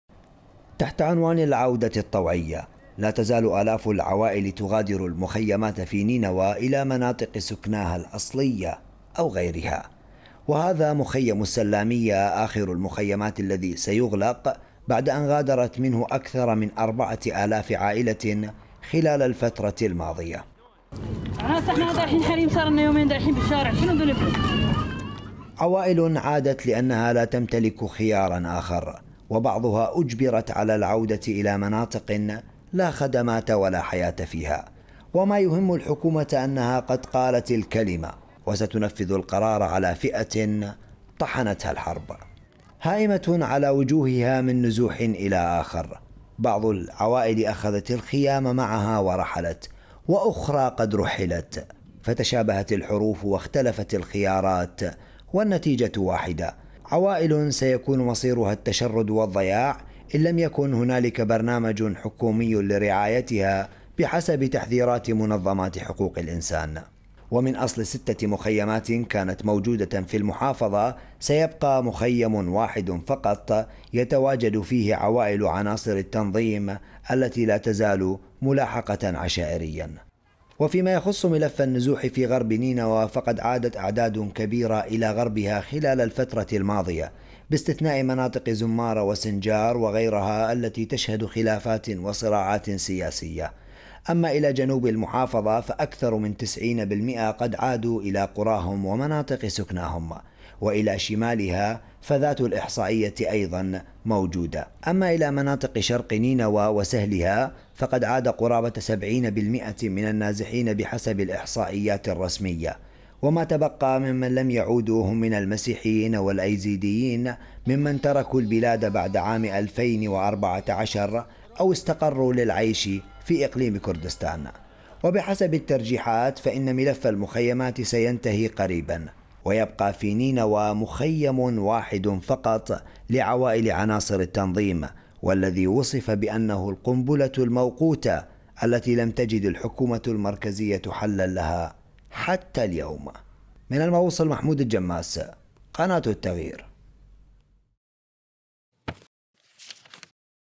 نازحو الموصل أصبحوا مشردين | تقرير
تنفيذاً لقرار إغلاق مخيمات النازحين في العراق، غادرت عشرات العائلات مخيم السلامية قرب مدينة الموصل، ضمن دفعة جديدة من النازحين الذين انقطعت بهم السبل جراء عدم إعادة إعمار منازلهم المتضررة جراء الحرب على داعش، خاصة مع عدم قدرتهم المالية على تحمل تكاليف استئجار بيوت بديلة..